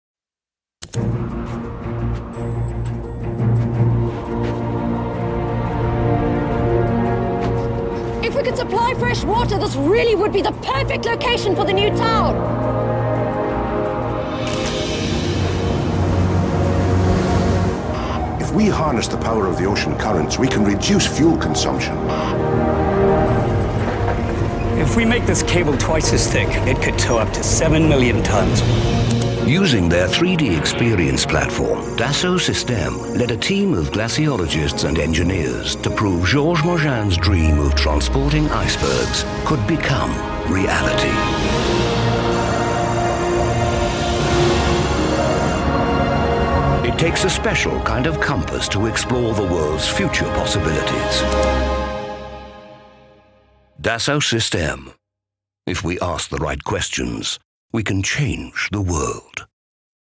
IceDream - IFWE by Dassault Systèmes
IF WE harvest icebergs, just one could provide half a million people with fresh water for a year. scientific TV documentary